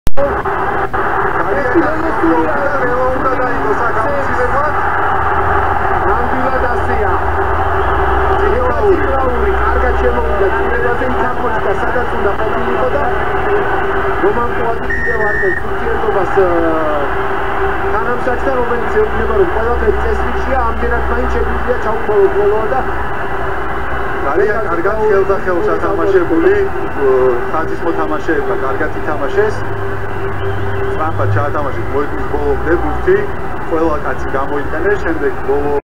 GEORGIA 35:24 JAPAN Fifth try (Beka Tsiklauri)
საქართველო 35:24 იაპონია ,ბექა წიკლაურის ლელო